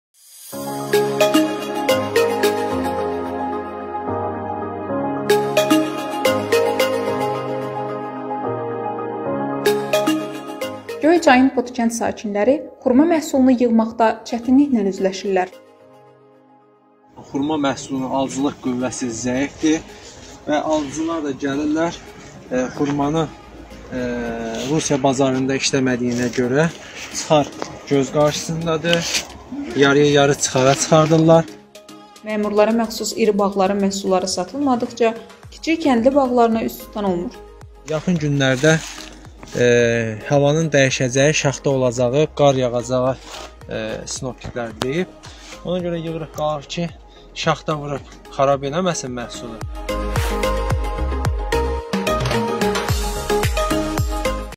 Göyçayın Potu kəndindən olan fermer 30 tondan artıq xurma meyvəsini sata bilməməsi barədə şikayət
Göyçayın Potu kəndindən olan fermer  30 tondan artıq xurma meyvəsini sata bilməməsindən şikayətlənir.
Məhsulu xarici bazarlara çıxarda bilmirlər.
Bağda yerə sərilmiş meyvənin isə bir müddət sonra xarab olacağını deyir.